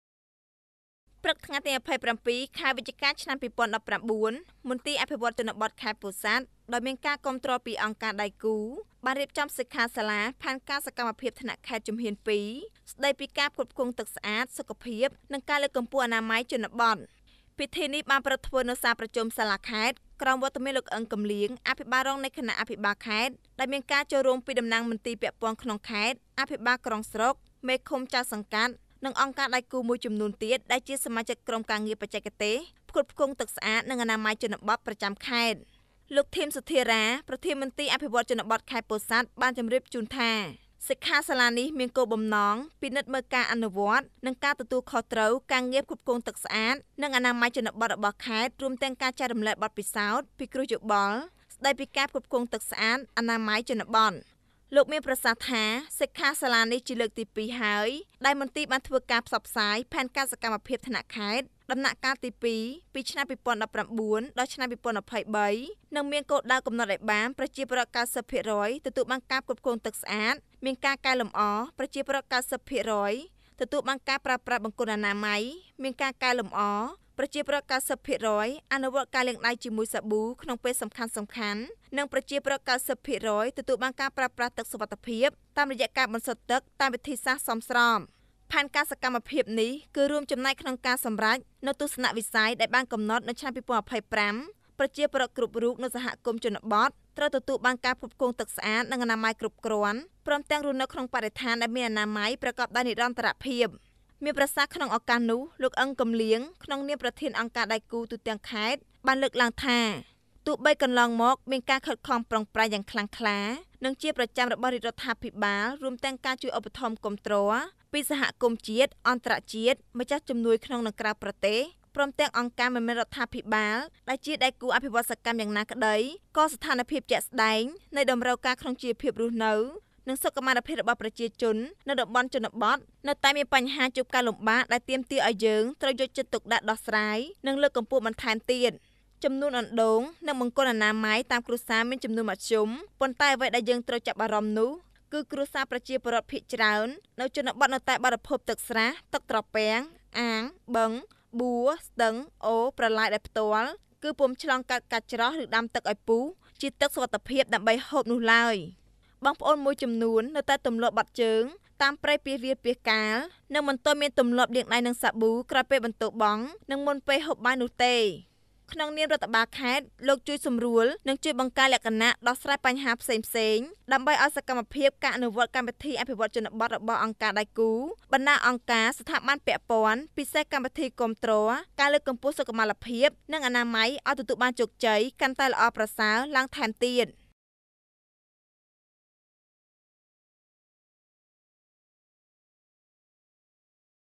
សិក្ខាសាលា ផែនការសកម្មភាពថ្នាក់ខេត្តជំហាន២ ស្ដីពីការផ្គត់ផ្គង់ទឹកស្អាត សុខភាព
ព្រឹកថ្ងៃទី២៧ខែវិច្ឆិការឆ្នាំ២០១៩ មន្ទីរអភិវឌ្ឍន៍ជនបទខេត្តពោធិ៍សាត់ ដោយមានការគាំទ្រពីអង្គការដៃគូរ បានរៀបចំសិក្ខាសាលា (ផែនការសកម្មភាពថ្នាក់ខេត្តជំហាន២ ស្ដីពីការផ្គត់ផ្គង់ទឹកស្អាត សុខភាព និងការលើកកម្ពស់អនាម័យជនបទ)។ ពិធីនេះប្រារព្វធ្វើនៅសាលប្រជុំសាលាខេត្ត ក្រោមវត្តមានលោក អ៊ឹង គឹមលាង អភិបាលរងនៃគណៈអភិបាលខេត្ត និងមានការចូលរួមពីតំណាងមន្ទីរពាក់ព័ន្ធក្នុងខេត្ត, អភិបាលក្រុងស្រុក, មេឃុំ ចៅសង្កាត់ និងអង្គការដៃគូរមួយចំនួនទៀត ដែលជាសមាជិកក្រុមការងារបច្ចេកទេសផ្គត់ផ្គង់ទឹកស្អាត និងអនាម័យជនបទប្រចាំខេត្ត។
 លោក ធីម សុធារ៉ា ប្រធានមន្ទីរអភិវឌ្ឍន៍ជនបទខេត្តពោធិ៍សាត់បានជម្រាបជូនថា សិក្ខាសាលានេះមានគោលបំណង ពិនិត្យមើលការអនុវត្តន៍និងការទទួលខុសត្រូវការងារផ្គត់ផ្គង់ទឹក និងអនាម័យជនបទរបស់ខេត្ត រួមនឹងការចែករំលែកបទពិសោធន៍ពិគ្រោះយោបល់ ស្ដីពីការផ្គត់ផ្គង់ទឹកនិងអនាម័យជនបទ។ លោកមានប្រសាសន៍ថា សិក្ខាសាលានេះជាលើកទី២ហើយ ដែលមន្ទីរបានធ្វើការផ្សព្វផ្សាយផែនការសកម្មភាពថ្នាក់ខេត្តដំណាក់កាលទី២ ពីឆ្នាំ២០១៩ដល់ឆ្នាំ២០២៣ នឹងមានគោលដៅកំណត់ឲ្យបាន ប្រជាពលរដ្ឋ៩០%ទទួលបានការផ្គត់ផ្គង់ទឹកមានការកែលម្អរ, ប្រជាពលរដ្ឋ៩០%ទទួលបានការប្រើប្រាស់បង្គន់មានការកែលម្អរ, ប្រជាពលរដ្ឋ៩០%អនុវត្តការលាងដៃជាមួយសាប៊ូក្នុងពេលសំខាន់ៗ និងប្រជាពលរដ្ឋ៩០%ទទួលបានការប្រើប្រាស់ទឹកសុវត្ថិភាពតាមរយៈការបន្សុទ្ធទឹកតាមវិធីសាស្ត្រសមស្រប។ ផែនការសកម្មភាពនេះនឹងចូលរួមចំណែកក្នុងការសម្រេចនូវទស្សនៈវិស័យដែលបានកំណត់នៅឆ្នាំ២០២៥ ប្រជាពលរដ្ឋគ្រប់រូបនៅសហគមន៍ជនបទ ត្រូវទទួលការផ្គត់ផ្គង់ទឹកស្អាតនិងអនាម័យគ្រប់គ្រាន់ ព្រមទាំងរស់នៅក្នុងបរិស្ថានដែលមានអនាម័យប្រកបដោយនិរន្តរភាព។
 មានប្រសាសន៍ក្នុងឱកាសនោះលោក អ៊ឹង គឹមលាង ក្នុងនាមប្រធានអង្គការដៃគូរទូទាំងខេត្តពោធិ៍សាត់បានលើកឡើងថា ទោះបីកន្លងមកមានការខិតខំប្រឹងប្រែងយ៉ាងខ្លាំងក្លានិងជាប្រចាំរបស់រាជរដ្ឋាភិបាល រួមទាំងការជួយឧបត្ថម្ភគាំទ្រពីសហគមន៍ជាតិ អន្តរជាតិ ម្ចាស់ជំនួយក្នុងនិងក្រៅប្រទេស ព្រមទាំងអង្គការមិនមែនរដ្ឋាភិបាលជាដៃគូរអភិវឌ្ឍន៍ដ៍សកម្មយ៉ាងណាក្ដី ក៍ស្ថានភាពជាក់ស្ដែងនៃតម្រូវការក្នុងជីវភាពរស់នៅ និងសុខមាលភាពរបស់ប្រជាជននៅតំបន់ជនបទ នៅតែមានបញ្ហាជួបការលំបាកដែលទាមទារឲ្យយើងត្រូវយកចិត្តទុកដាក់ដោះស្រាយ និងលើកកម្ពស់បន្ថែមទៀត។ ចំនួនអណ្ដូងនិងបង្គន់អនាម័យតាមគ្រួសារមានចំនួនមធ្យម ប៉ុន្តែអ្វីដែលយើងត្រូវចាប់អារម្មណ៍នោះគឺ គ្រួសារប្រជាពលរដ្ឋភាគច្រើននៅជនបទ នៅតែបរិភោគទឹកស្រះ ទឹកត្រពាំង អាង បឹងបួរ ស្ទឹងអូរ ប្រឡាយដោយផ្ទាល់ គឺពុំបានឆ្លងកាត់ការច្រោះឫដាំឲ្យពុះទៅជាទឹកសុវត្ថិភាពដើម្បីហូបនោះឡើយ។បងប្អូនមួយចំនួននៅតែទម្លាប់បត់ជើងបត់ជើងតាមព្រៃពាសវាលពាសកាល និងមិនទាន់មានទម្លាប់លាងដៃនឹងសាប៊ូក្រោយពេលបន្ទោបង់ និងមុនពេលហូបបាយនោះទេ។ ក្នុងនាមរដ្ឋបាលខេត្តលោកនឹងជួយសម្របសម្រួលបង្កលក្ខណៈដោះស្រាយបញ្ហាផ្សេងៗ ដើម្បីឲ្យឲ្យសកម្មភាពការអនុវត្តកម្មវិធីអភិវឌ្ឍន៍ជនបទរបស់បណ្ដាអង្គការ ស្ថាប័នពាក់ព័ន្ធ ពិសេសកម្មវិធីគាំទ្រដល់ការលើកកម្ពស់សុខភាព និងអនាម័យឲ្យទទួលបានជោគជ័យ កាន់តែល្អប្រសើរឡើងថែមទៀត៕ សែម សុគន្ធ